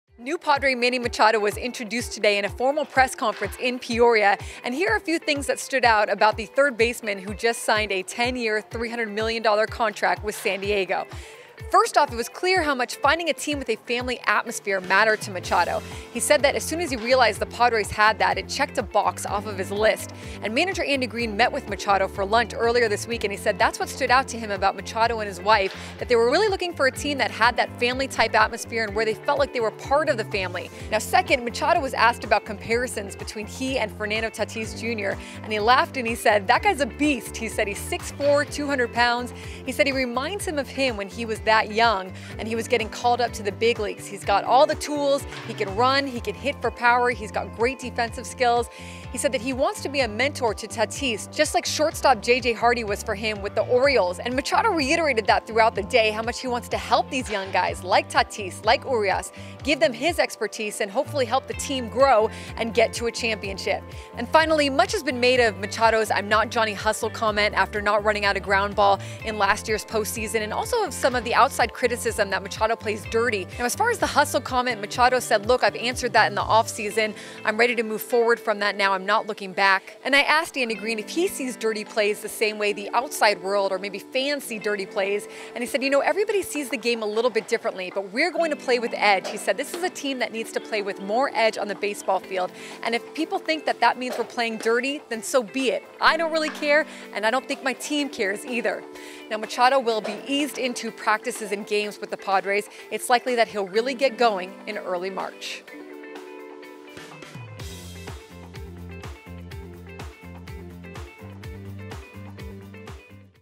Three Things from Manny Machado's First Day With Padres | San Diego Union-Tribune
What stood out from superstar third baseman Manny Machado's first day with the Padres? Watch and find out.